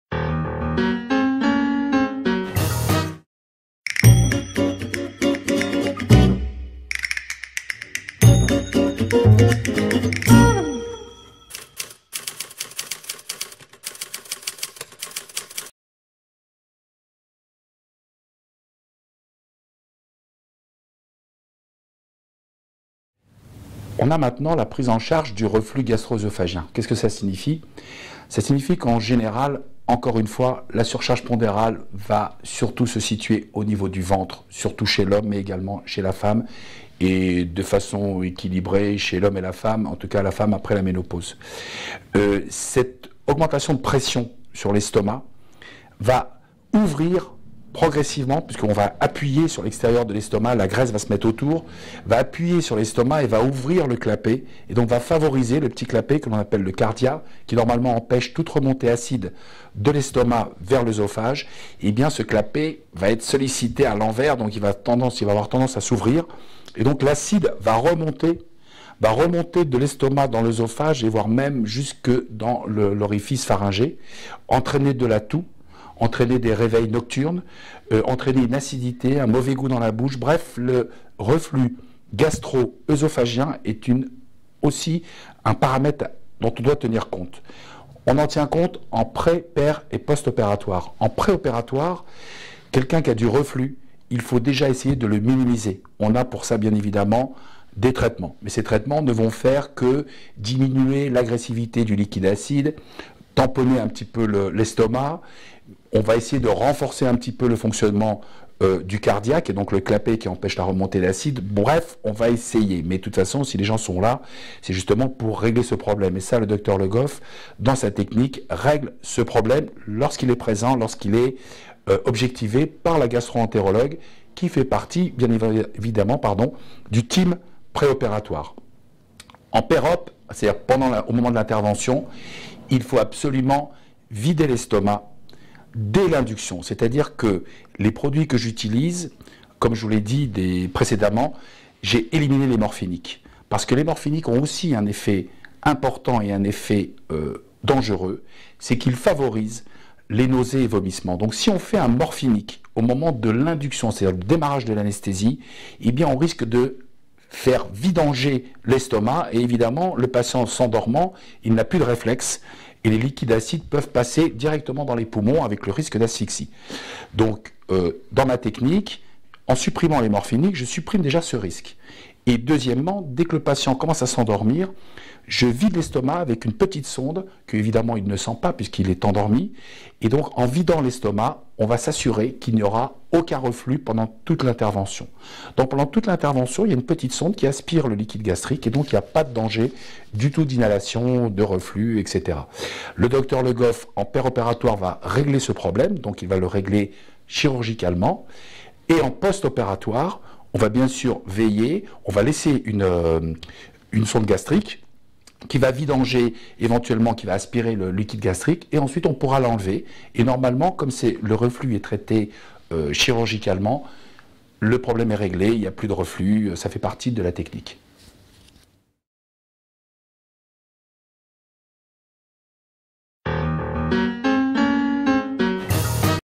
METHODE LE GOFF : Anesthésie & Risques Digestifs (Chirurgie bariatrique)
FILM :  Rôle de l'Anesthésiste dans la Méthode Le Goff

EPISODE 4 :  Anesthésie & Risques Digestifs
(Cas du Reflux Gastro-Oesophagien)

Interview du Dr. Guedj Jean-Yves
Anesthésiste-Réanimateur